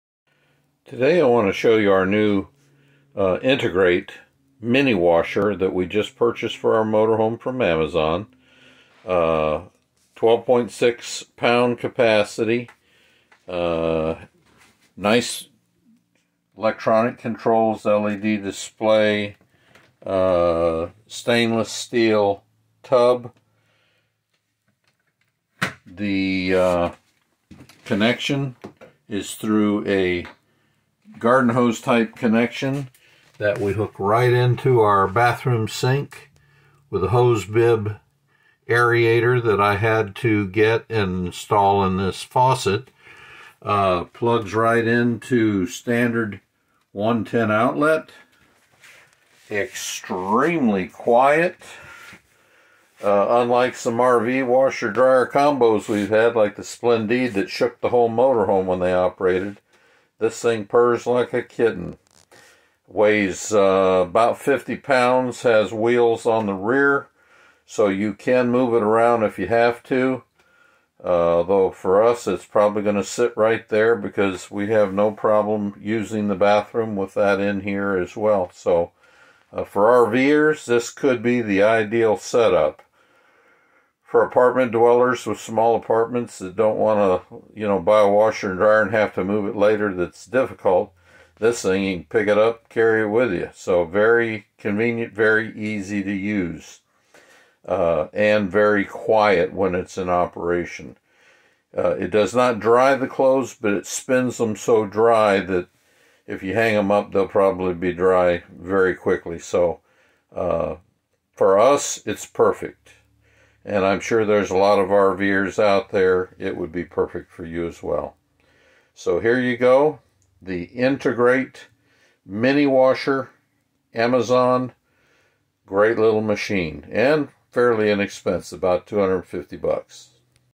Intergreat Mini Washer
12.6 lb mini washer. 110vac hookup. Connect to a sink or W/D hookups, and drain to sink or W/D drain if available. Quiet, inexpensive, easy to move around,  and spins clothes nearly dry.